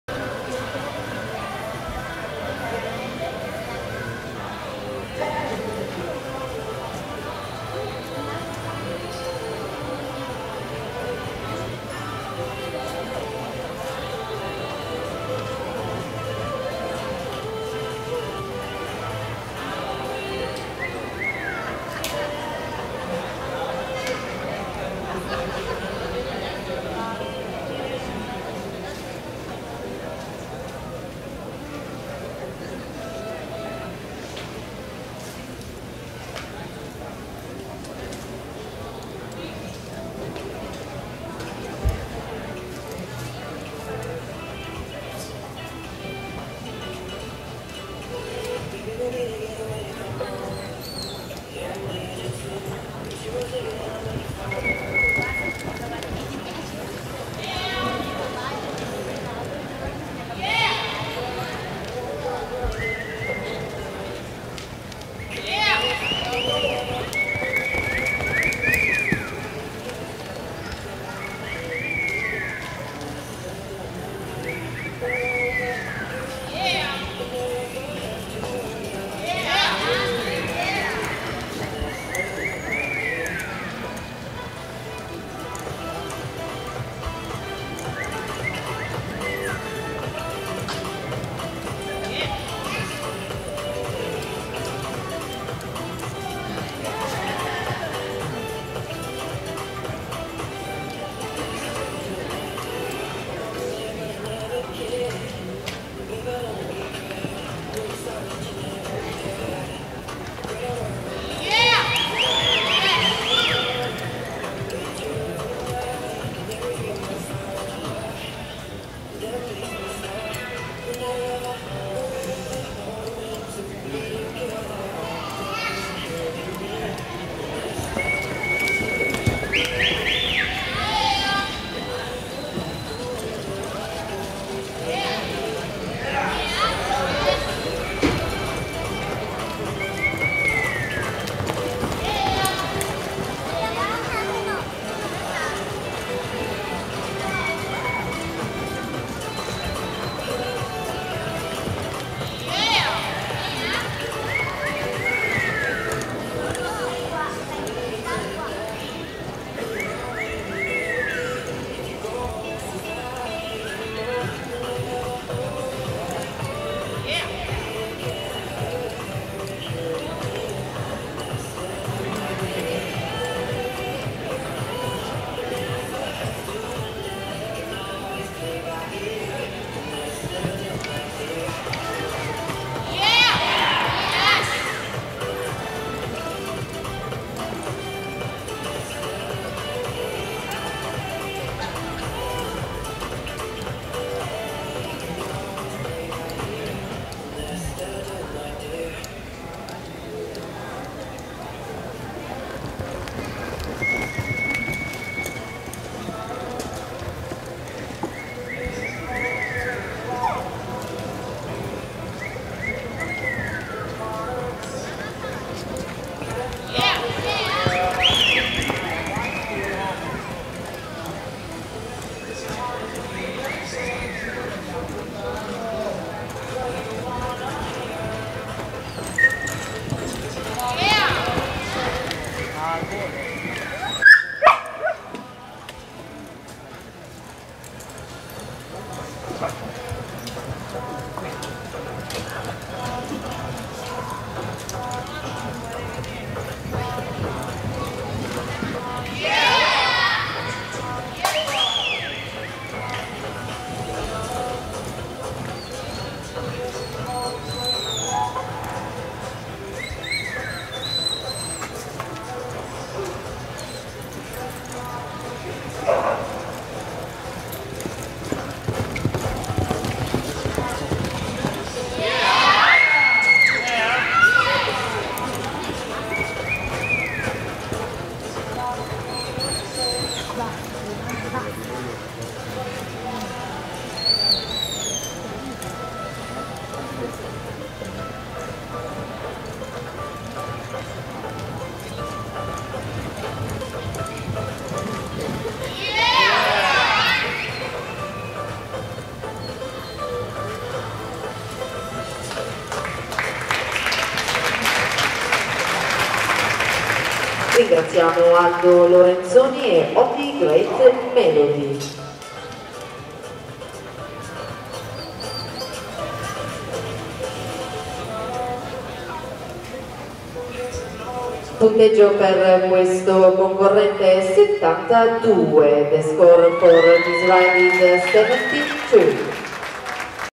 NRHA BACK TO THE FUTUR...ITY APCR 2018 - LORENZONI ALDO su OT GREAT MELODY con score 72
LORENZONI ALDO su OT GREAT MELODY di GEROMIN FEDERICA con score 72 - 1 classificato APCR FUTURITY 3 YRS OLD INT. OPEN e APCR FUTURITY 3 YRS OLD OPEN - NRHA BACK TO THE FUTUR...ITY APCR 2018 - credits Studio di fotografia Andrea Bonaga